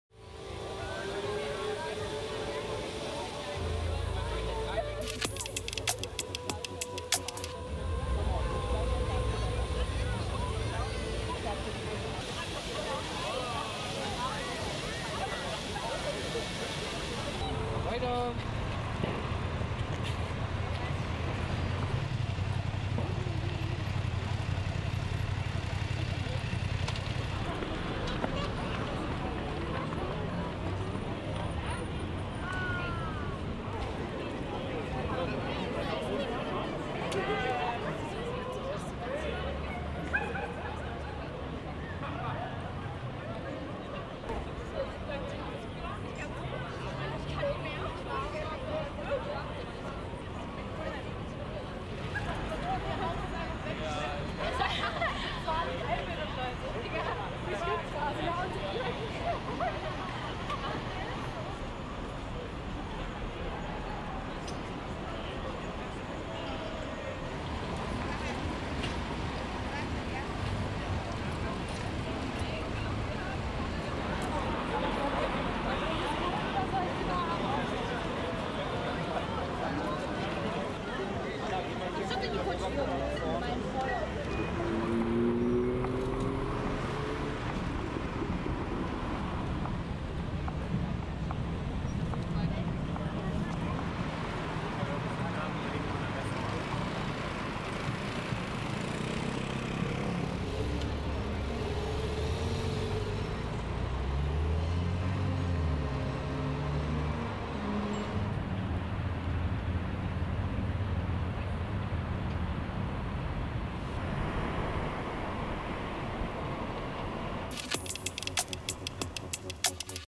تصاویری از جشنواره نور در شهر برلین
جشنواره نور برلین امسال با موضوع «ساختن آینده» از روز جمعه سوم سپتامبر برای هفدهمین بار در پایتخت آلمان برپا شده  و تا سیزدهم ماه جاری به صورت رایگان میزبان بازدیدکنندگان است.